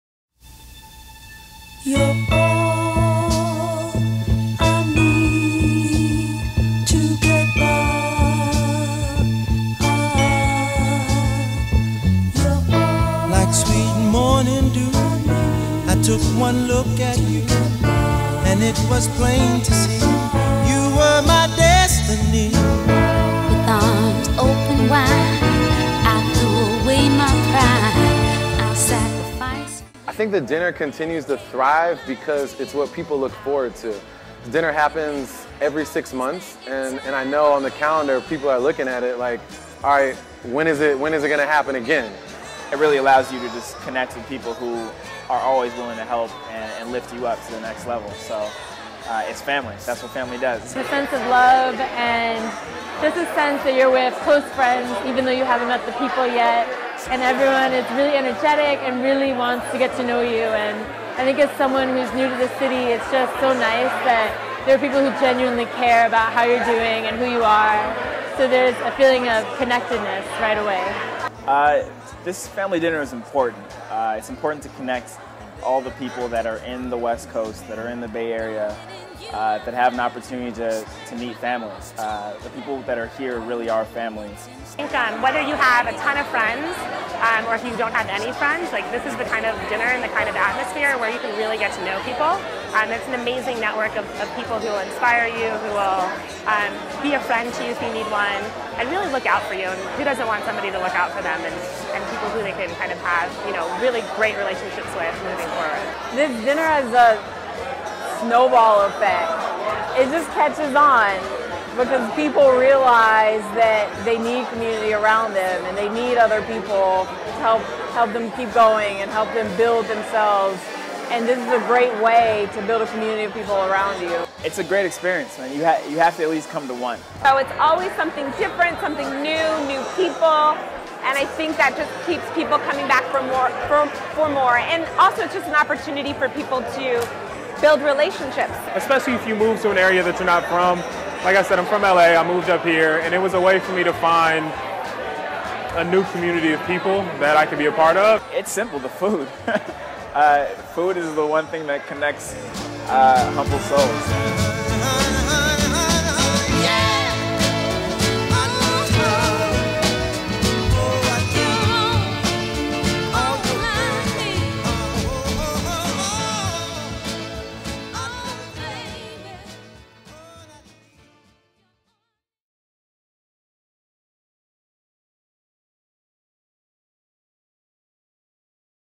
The Family Dinner - ourfamilydinner.org - Preview Trailer
The Family Dinner - ourfamilydinner.org - Preview Reel I

Founded in New York by Lawrence Adjah, the community-based family dinner tradition now hosts close to 2,000 of the nation's most influential, passionate and diverse young people, over family style dinners across three cities, New York, San Francisco and Dallas. In a few months, this dinner tradition will make its way to Houston and Washington, D.C.

What started out as a thirty-person dinner five years ago in New York has organically grown into a warm tradition with a sincere commitment to making people feel at home...at home. The Family Dinner is truly building a community, one dinner at a time.